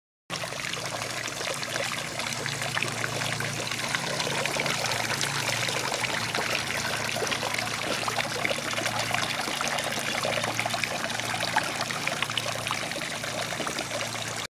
COPPER SWAN/FLOWER WATER FOUNTAIN
Selling Saturday, October 16th
All Proceeds (Including Buyer’s Premium) Goes to the
Alzheimer's Services earmarked for Charlie's Place in Gonzales.
Fountain and sugar kettle built and donated by Waterfall Forge in Prairieville as part of it's Making for Giving project.